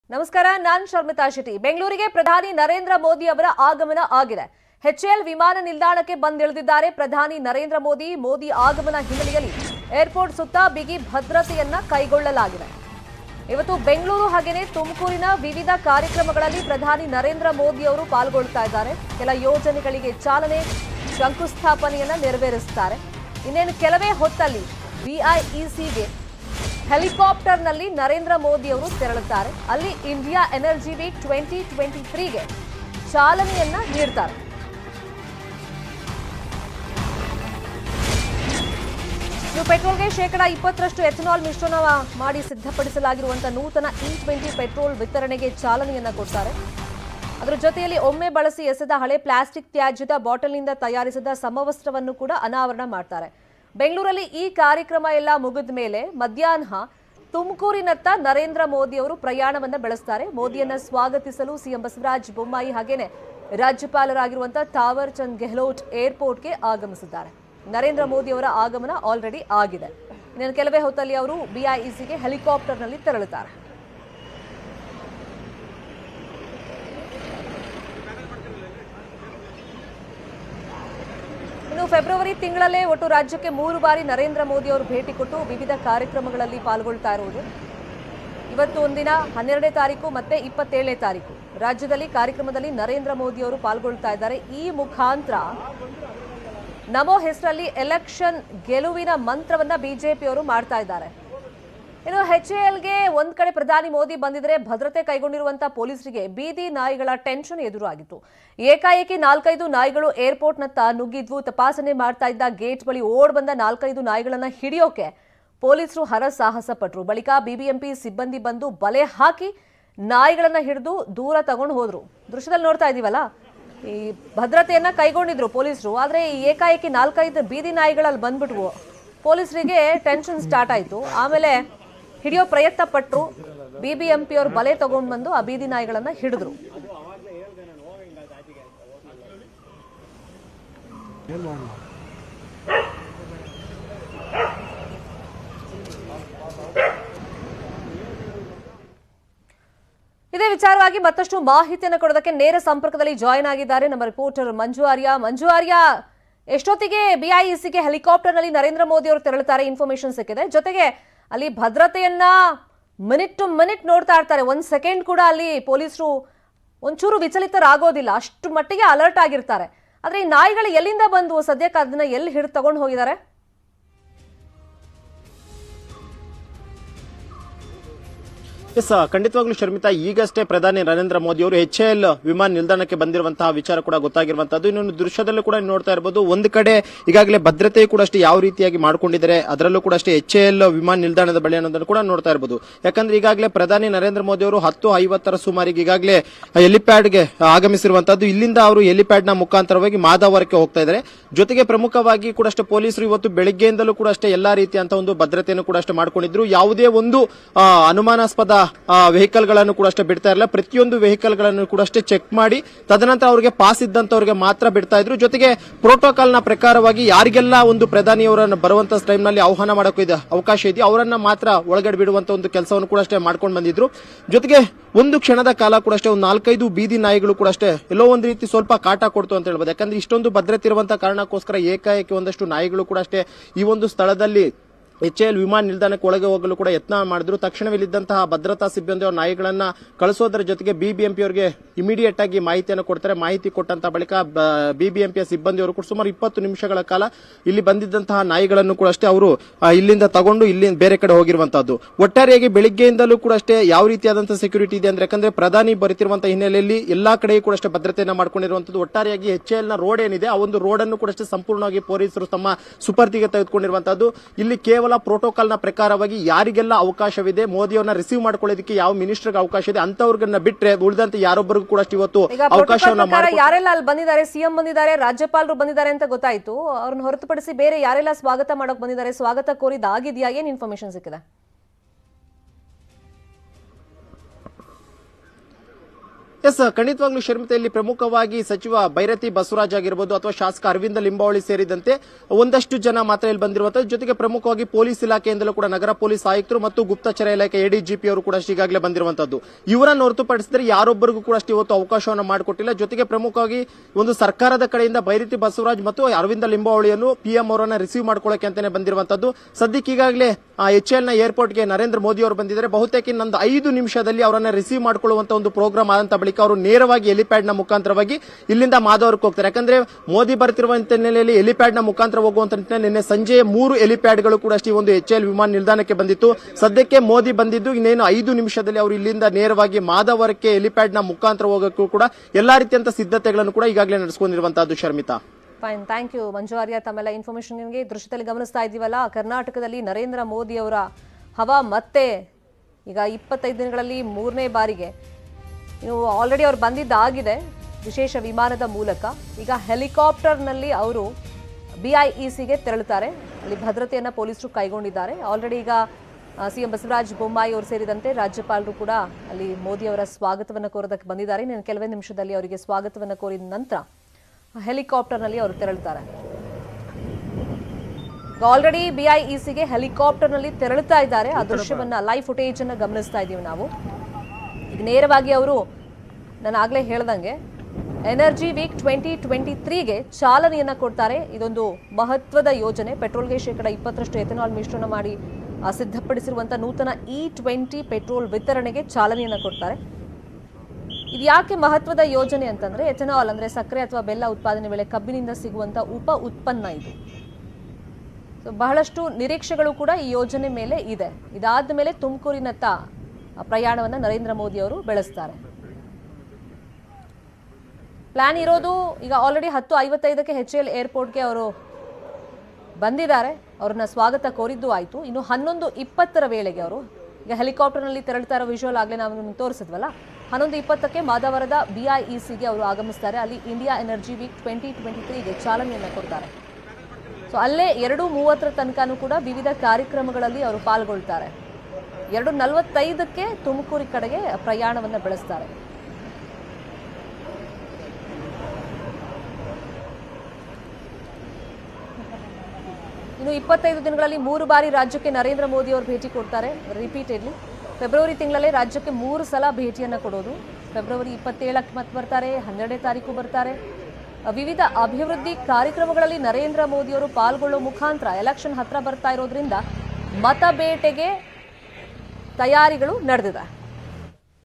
Narendra Modi Karnataka Visit | ನಮೋ ಭದ್ರತೆಗೆ ಬೀದಿನಾಯಿಗಳ ಕ್ವಾಟ್ಲೆ ! | Karnataka Election
Narendra Modi Karnataka Visit | ನಮೋ ಭದ್ರತೆಗೆ ಬೀದಿನಾಯಿಗಳ ಕ್ವಾಟ್ಲೆ ! ನರೇಂದ್ರ ಮೊದಿ ಕರ್ನಾಟಕಕ್ಕೆ ಭೇಟಿ ನೀಡುವ ಹಿನ್ನಲೆ ಬೆಂಗಳೂರಿನ ಹಚ್​ಎಲ್ ಏರ್​ಪೋರ್ಟ್ ಬಳಿ ಸಕಲ ಭದ್ರತೆ ವ್ಯವಸ್ಥೆ ಮಾಡಿದ್ದರು ಸಹ  ಬೀದಿನಾಯಿಗಳು ನುಗ್ಗಿದವು ನಂತರ ಬಿಬಿಎಂಪಿ ಸಿಬ್ಬಂದಿ ನಾಯಿಗಳನ್ನು ಹಿಡಿದಿದ್ದಾರೆ | Karnataka Election